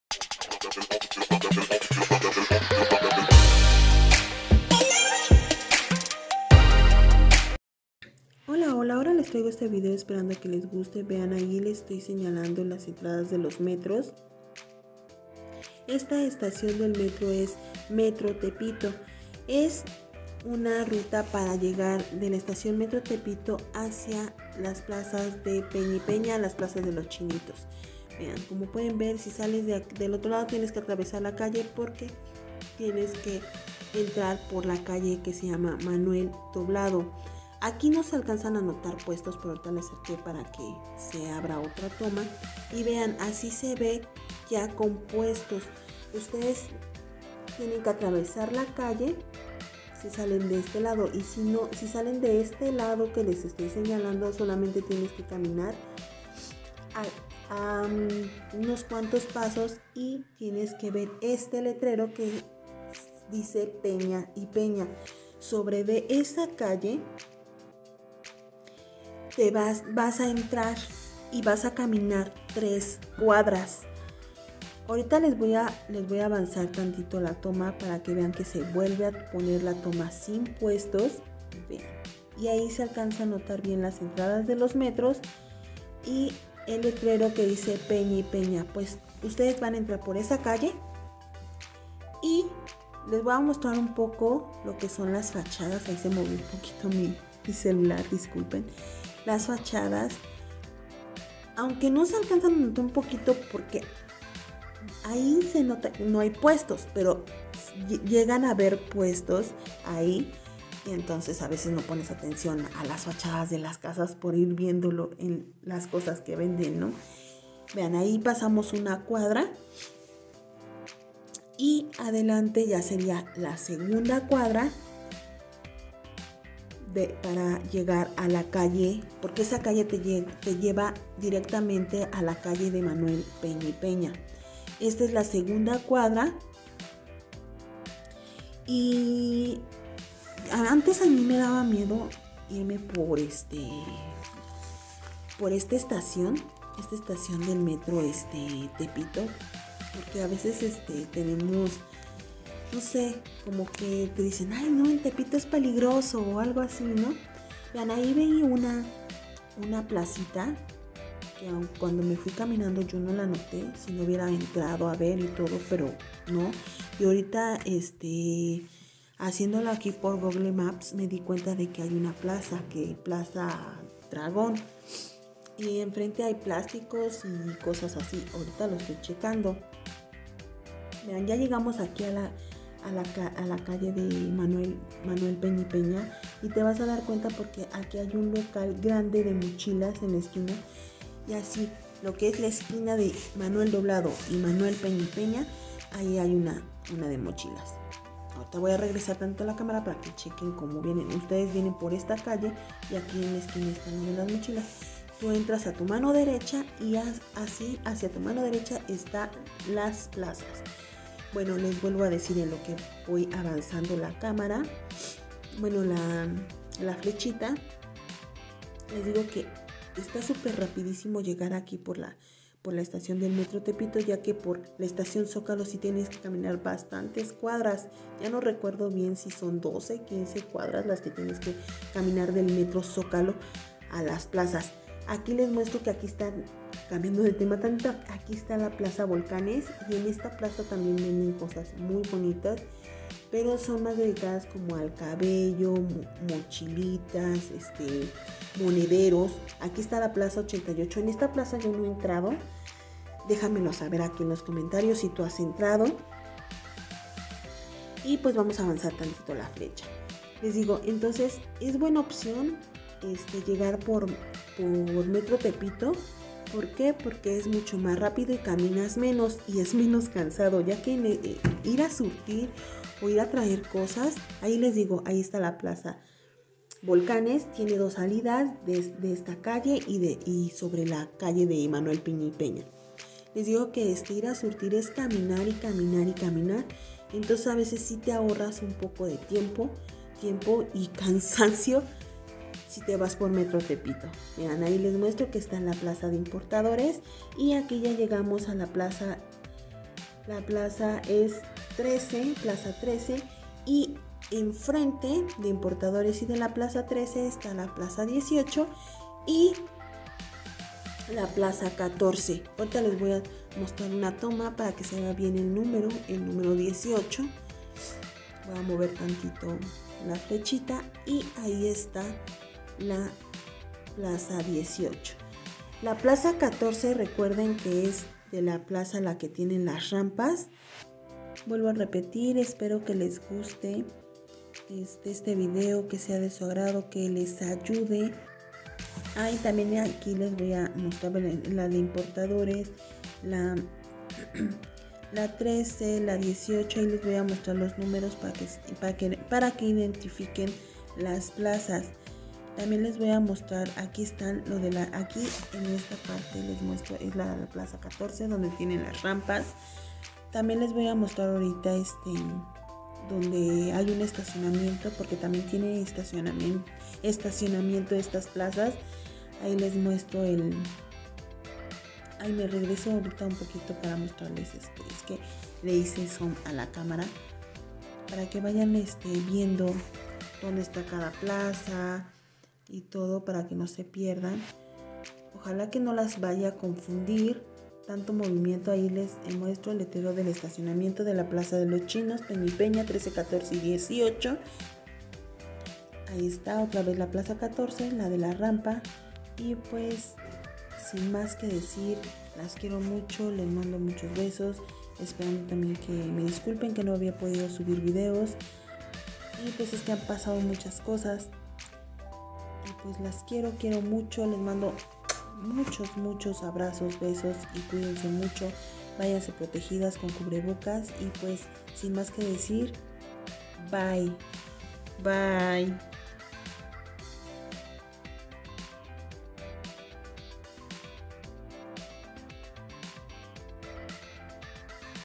🤩😁Cómo llegar alas plazas PEÑA Y PEÑA (Por METRO TEPITO)😁🤩🤪
llegando alas plazas por la estación del metro tepito